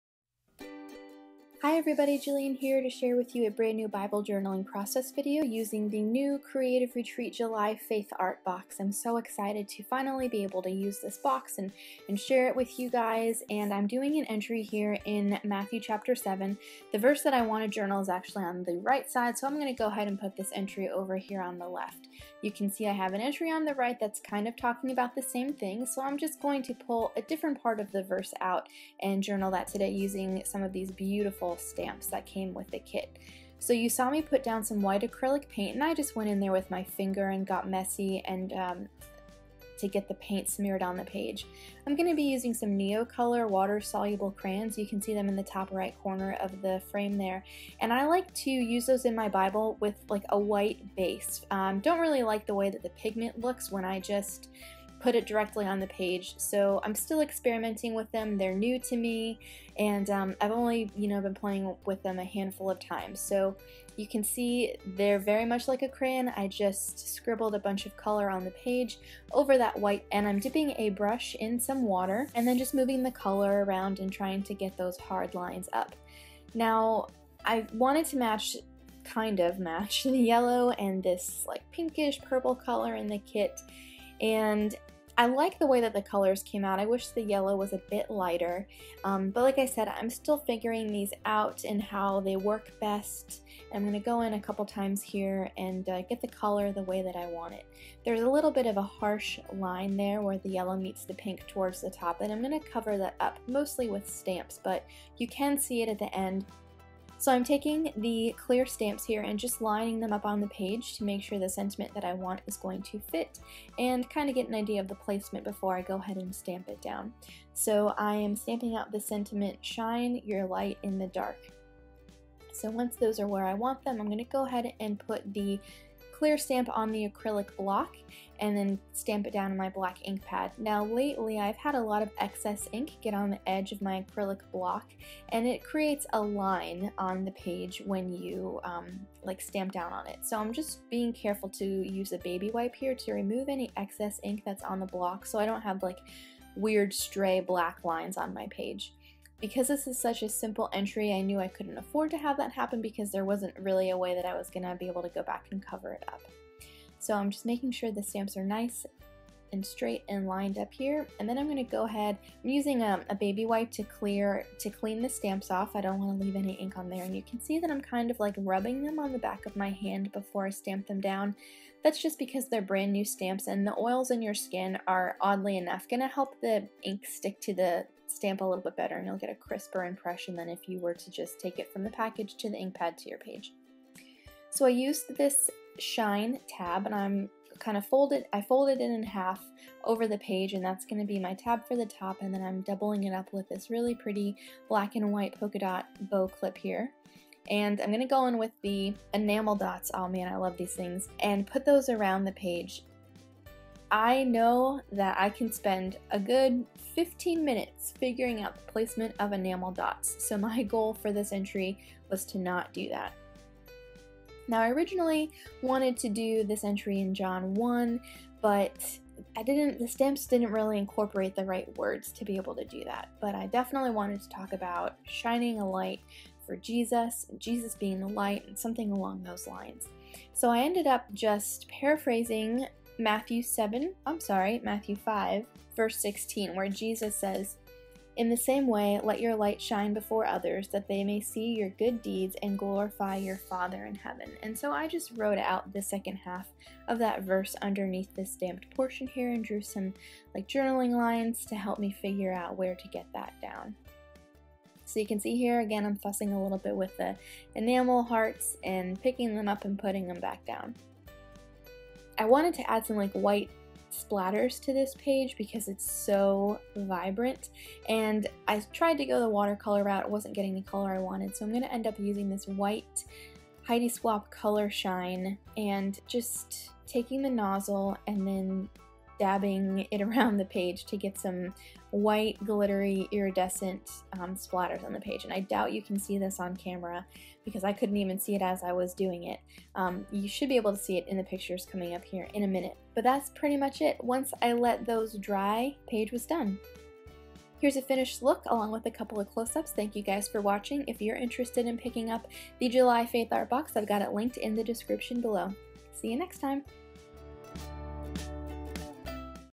Shine Your Light | July Faith Art Box
Bible journaling entry created by Hello Jillsky for Creative Retreat using the July Faith Art Box.

Be sure to check the Creative Retreat blog for bible journaling inspiration daily!